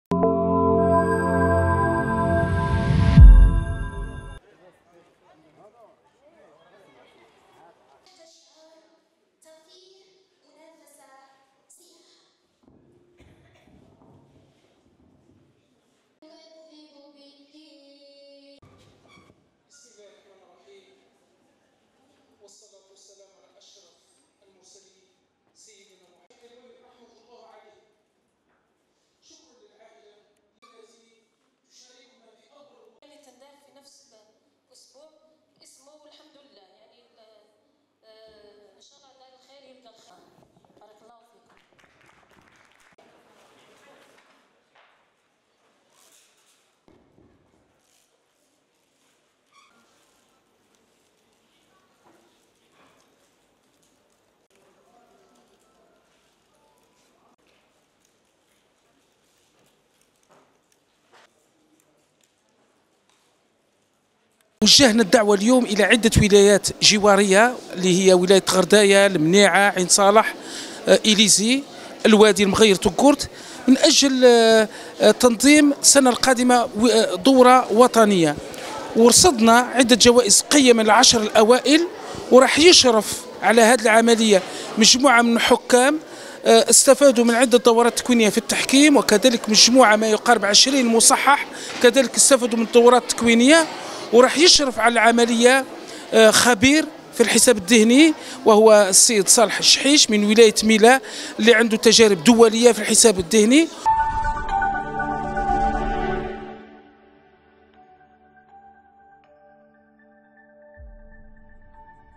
ورقلة : انطلاق البطولة الجهوية للحساب الذهني بمشاركة 8 ولايات
ورقلة (وأج) - شارك حوالي 90 طفلا من كلا الجنسين، تتراوح  أعمارهم ما بين سبع سنوات و14 سنة، في فعاليات البطولة الجهوية للحساب الذهني  (السوروبان) التي نظمت بدار الشباب ''الشهيد مصطفى بن بولعيد'' بمدينة ورقلة. ويمثل المشاركون في هذه البطولة الجهوية، التي تقام ليوم واحد وحملت اسم  المرحوم "عبد القادر قيرواني" , ثماني ولايات بالمنطقة الجنوبية الشرقية من الوطن، وهي ورقلة والمنيعة والمغير وغرداية وتوقرت والوادي وايليزي وان صالح.